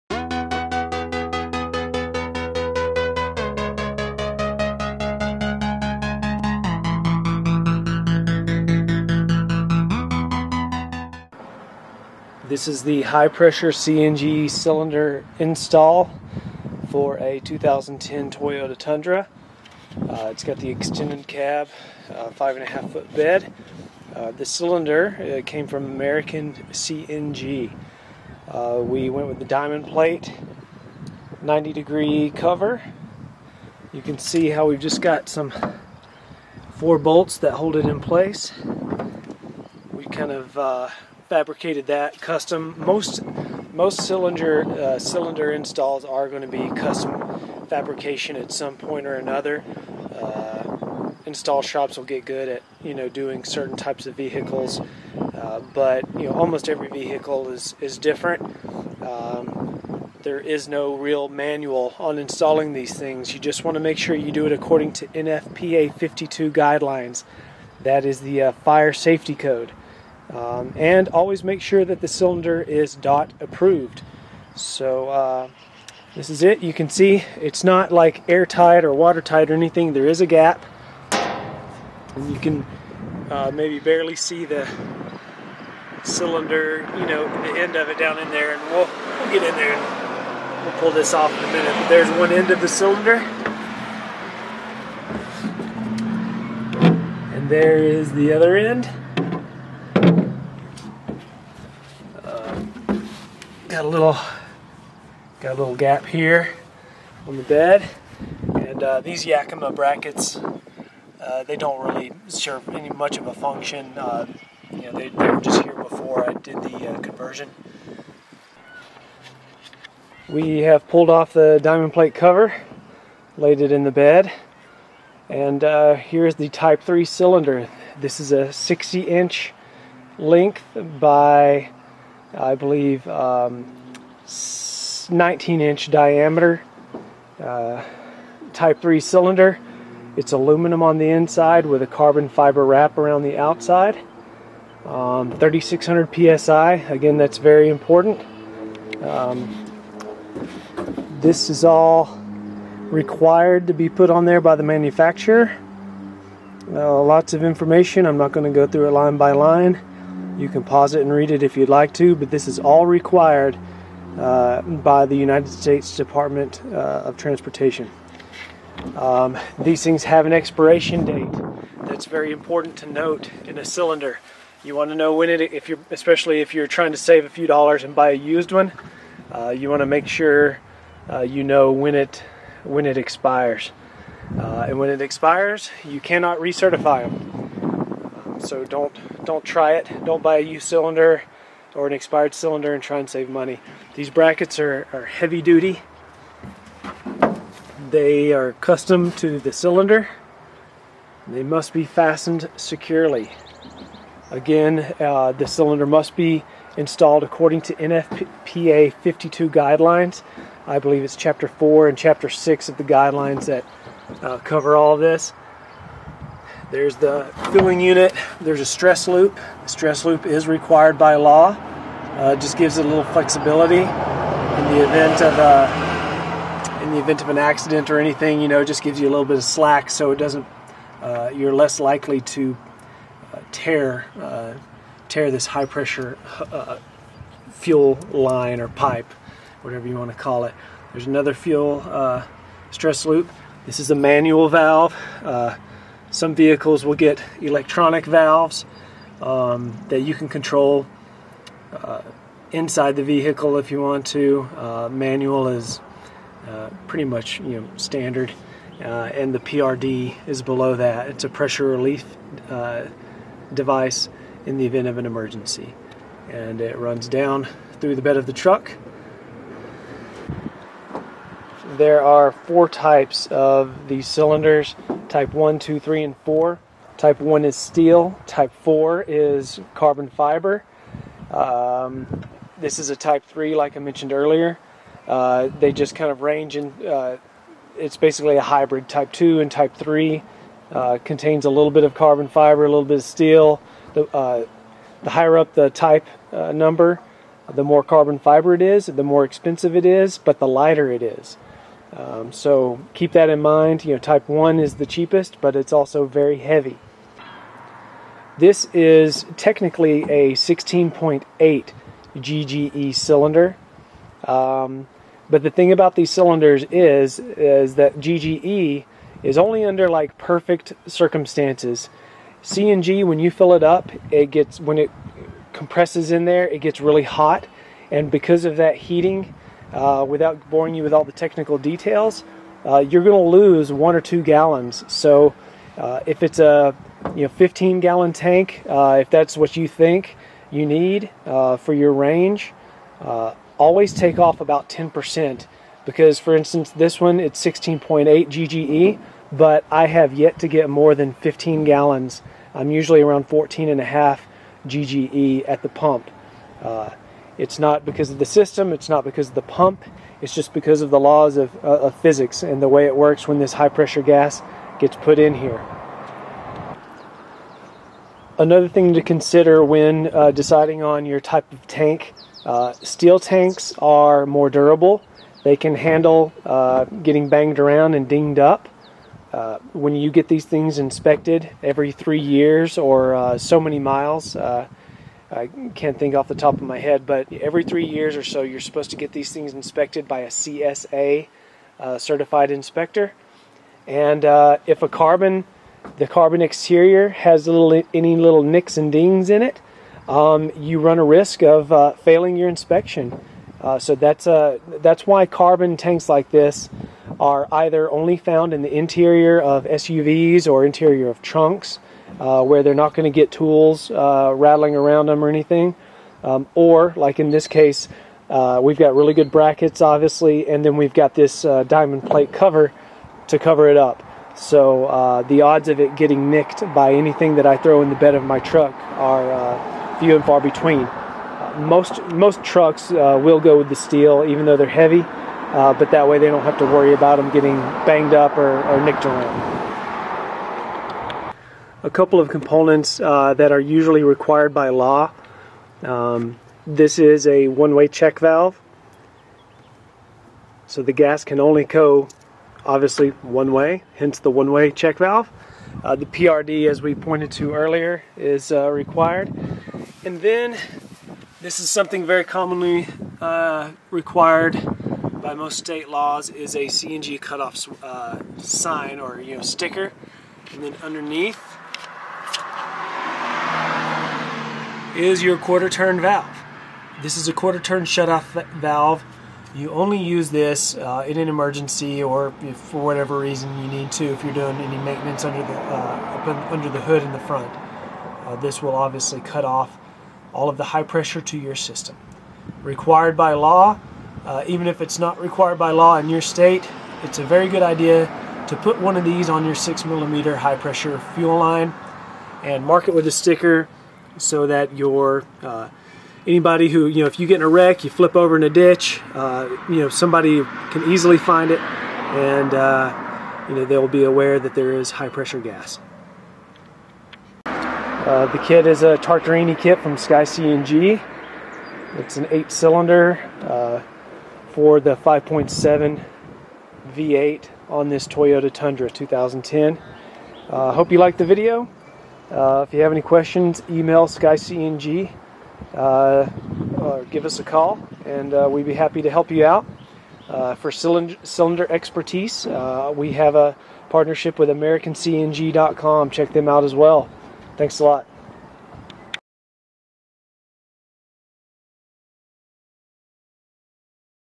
CNG Cylinder Tank CNG Conversion 2010 Toyota Tundra SkyCNG
CNG cylinder installation intro for 2010 Toyota Tundra. Type 3 CNG tank.